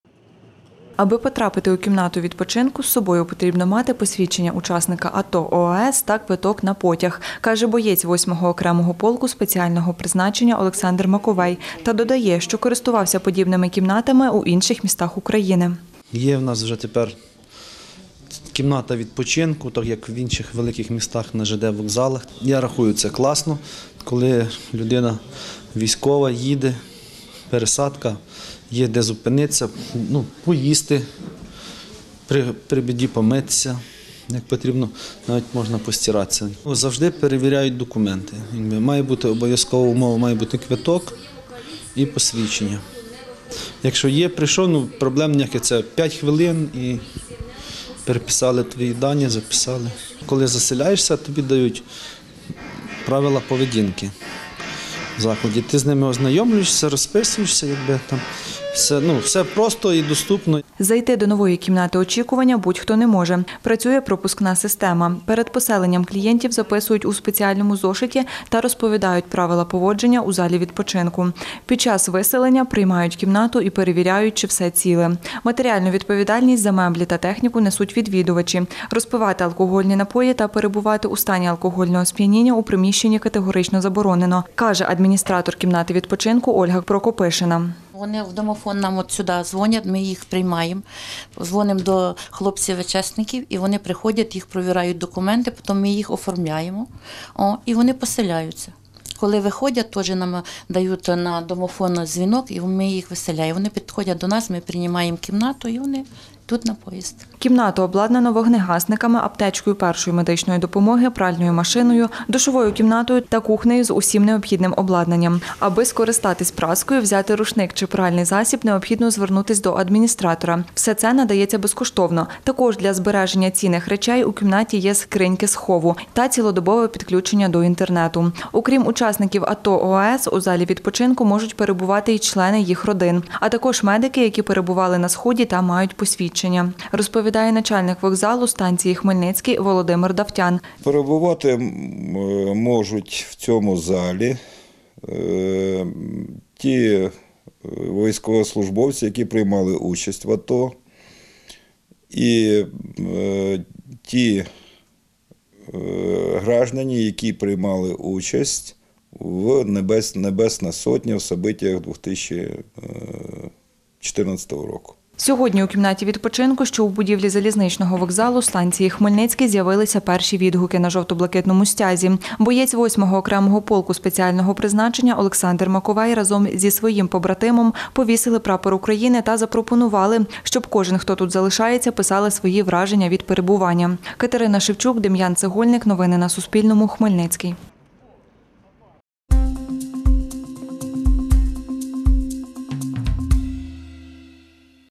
На Хмельницькому вокзалі військові можуть попрати та помитися
Окремий зал очікування для учасників АТО/ООС відкрили в східному крилі вокзалу станції Хмельницький. Про це кореспонденту Суспільного розповів начальник установи Володимир Давтян. Кімната, яка працює цілодобово, розрахована на 10 осіб.